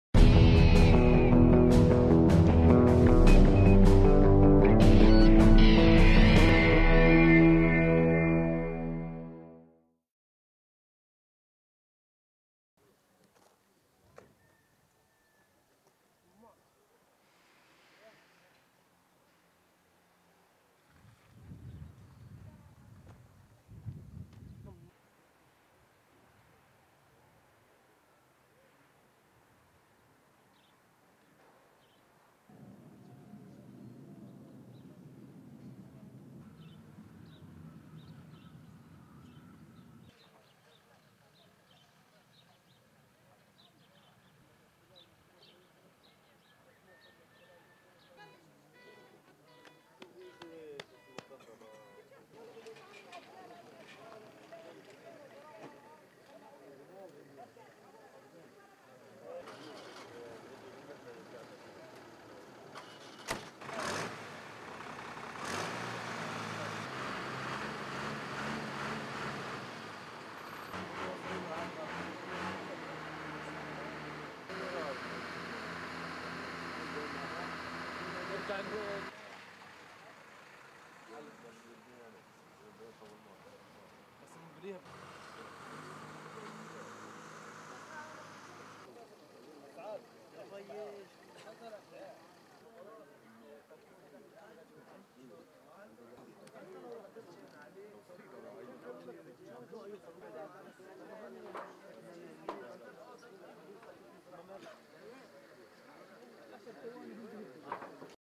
الاحتلال يهدم ورشة حدادة قرب قرية سردا شمال رام الله صباح اليوم الثلاثاء23-12-2014
Occupation destroys a metal workshop near the village of Surda, north of Ramallah on Tuesday morning 23/12/2014